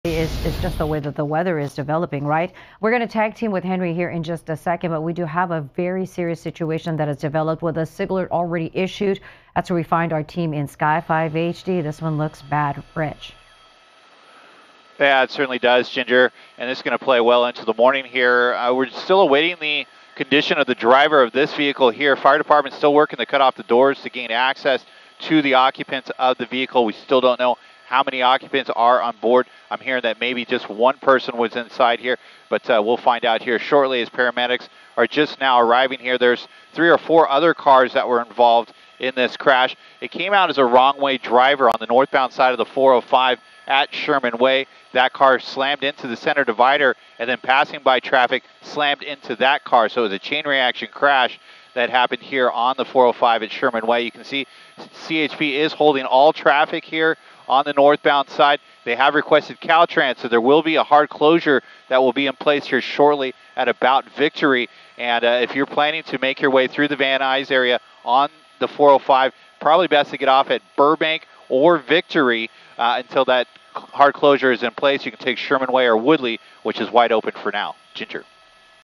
Multi-vehicle crash involving wrong way driver stops all traffic on 405 Freeway
A crash involving several vehicles shut part of the 405 Freeway down early Monday morning.
KTLA 5's Rich Prickett reports from Sky5.  (Oct. 30, 2023)